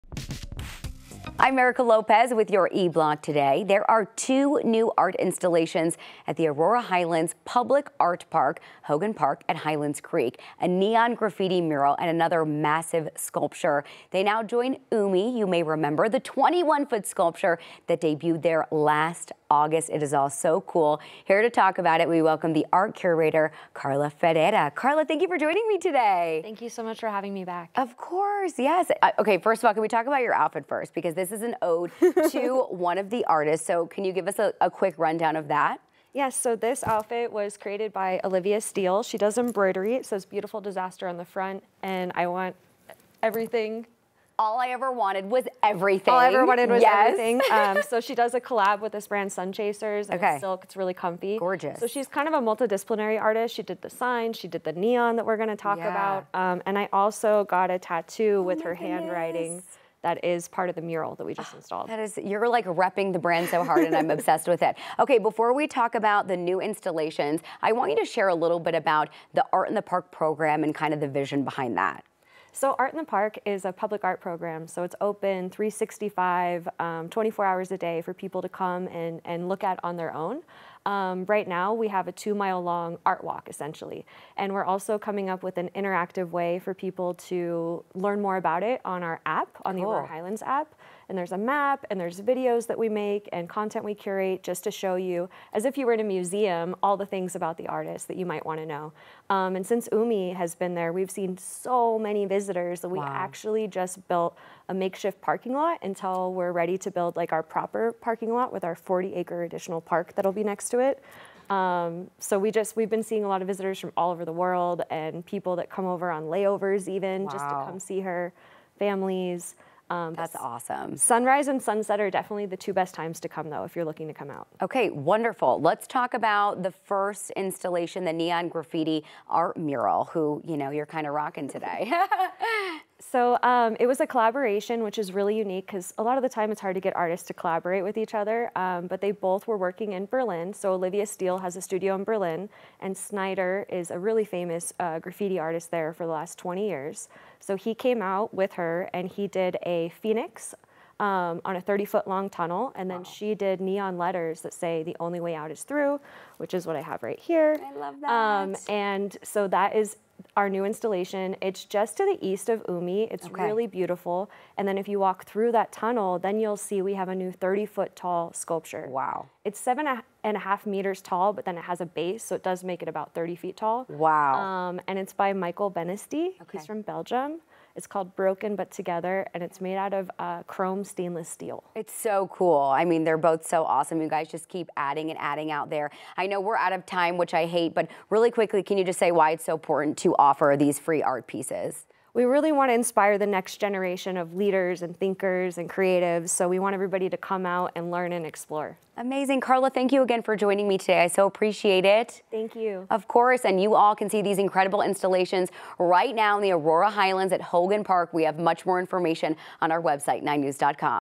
2 new installations added to Aurora Highlands public art park
The new installations can be see along a 2-mile art walk located at Hogan Park at Highlands Creek.

9NEWS (KUSA) is located in Denver, Colorado.